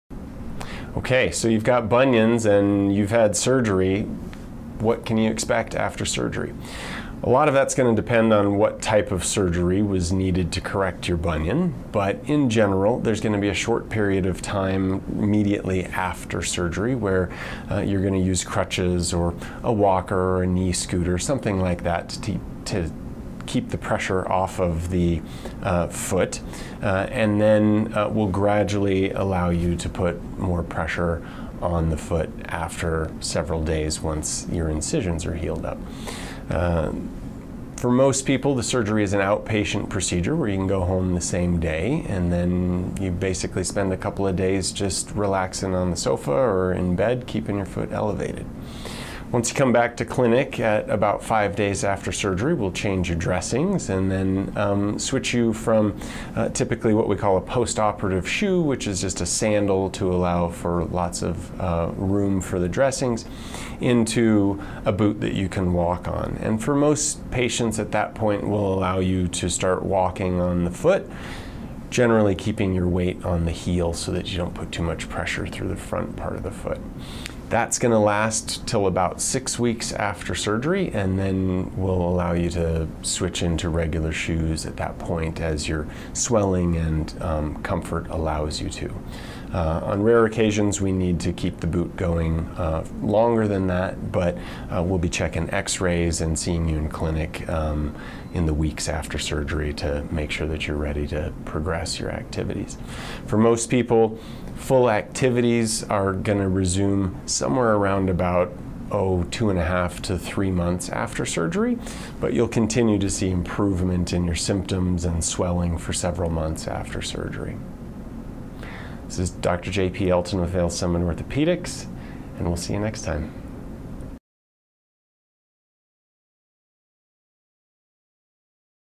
What to Expect After Bunion Surgery
When considering bunion surgery, it's important to understand what to expect afterward. In this video, Dr. Elton provides a detailed overview of the post-operative experience, from the initial healing process to when you can return to your regular activities.